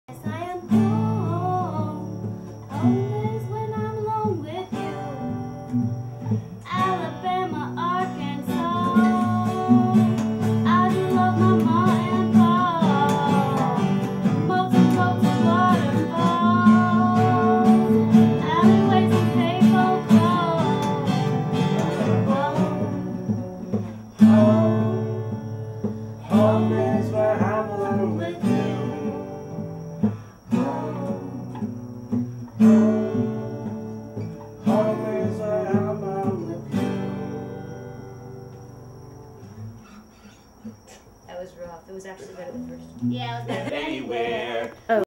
Black Friday - Well Done
A family plays music on a front porch rather than pepper spraying others for XBoxes.